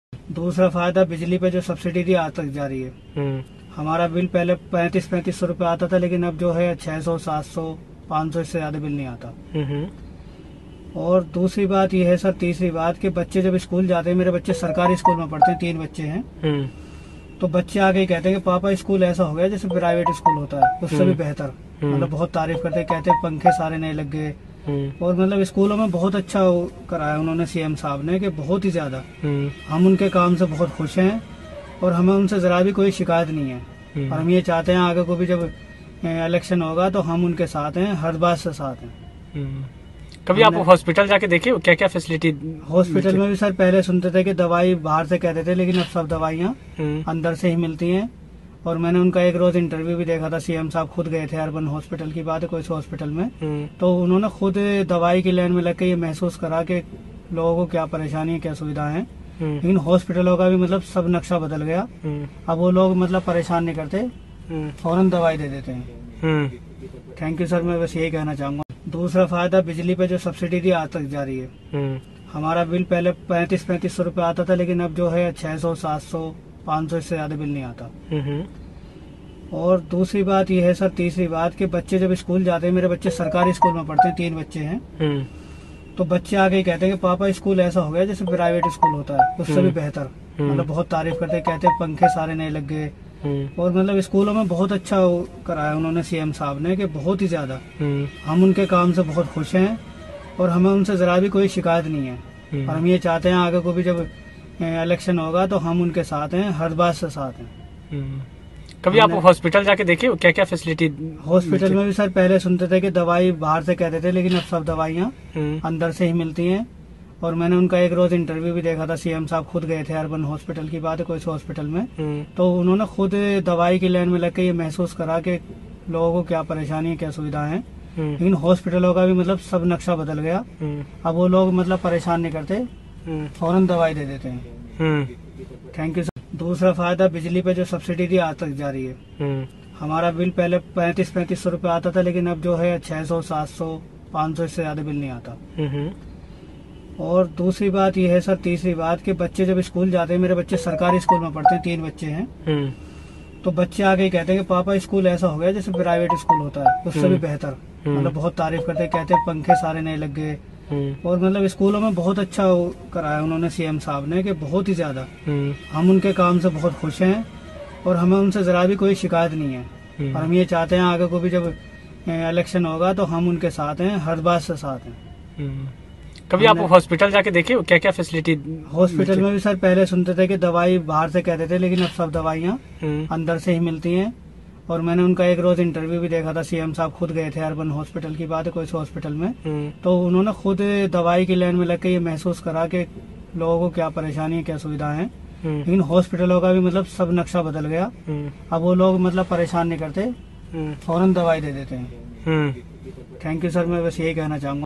"ਮੇਰੇ ਤਿੰਨੋਂ ਬੱਚੇ ਸਰਕਾਰੀ ਸਕੂਲ 'ਚ ਪੜ੍ਹਦੇ ਨੇ" ,ਬੱਚੇ ਕਹਿੰਦੇ ਪਾਪਾ ਸਰਕਰੀ ਸਕੂਲ ਹੁਣ ਪ੍ਰਾਈਵੇਟ ਨਾਲੋਂ ਵਧਿਆ ਨੇ
"ਮੇਰੇ ਤਿੰਨੋਂ ਬੱਚੇ ਸਰਕਾਰੀ ਸਕੂਲ 'ਚ ਪੜ੍ਹਦੇ ਨੇ" ,ਬੱਚੇ ਕਹਿੰਦੇ ਪਾਪਾ ਸਰਕਰੀ ਸਕੂਲ ਹੁਣ ਪ੍ਰਾਈਵੇਟ ਨਾਲੋਂ ਵਧਿਆ ਨੇ ,ਸੁਣੋ ਟੈਕਸੀ ਡਰਾਈਵਰ ਦੀਆਂ ਗੱਲਾਂ